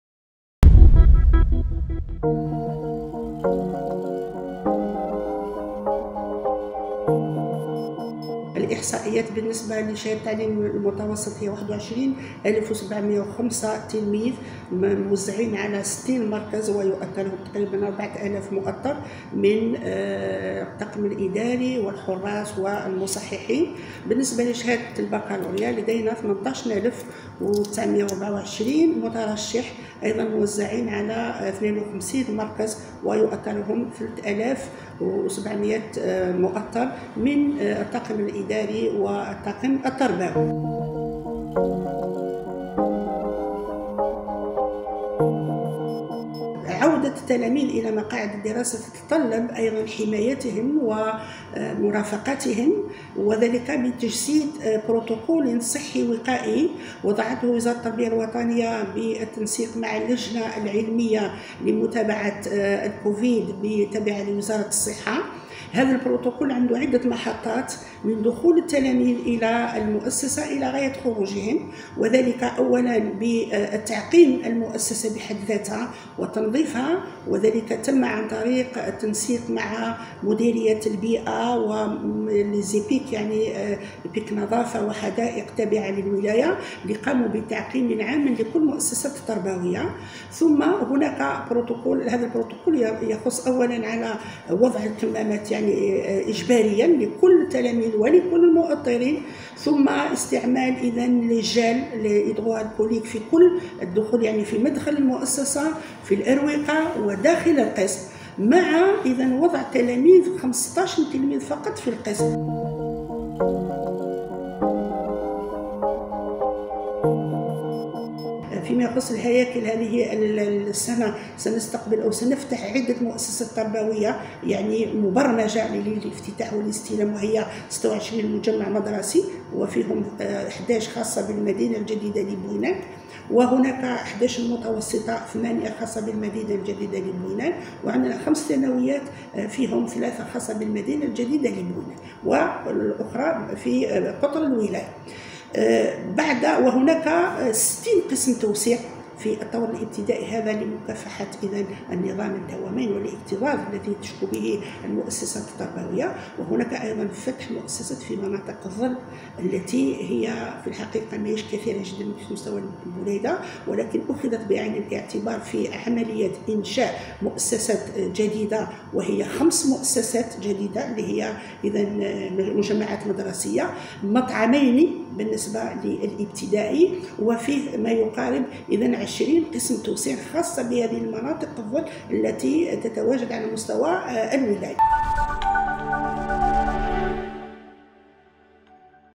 مديرة التربية للبليدة في حوار خاص لـ (وأج)
البليدة(وأج)- كشفت مديرة التربية غنيمة آيت إبراهيم في حوار خاص لـ وأج عن أهم الإجراءات المتخذة لإنجاح الموسم الدراسي القادم في ظل تفشي فيروس كورونا، وكذا أبرز ما يتعلق بامتحانات الأقسام النهائية.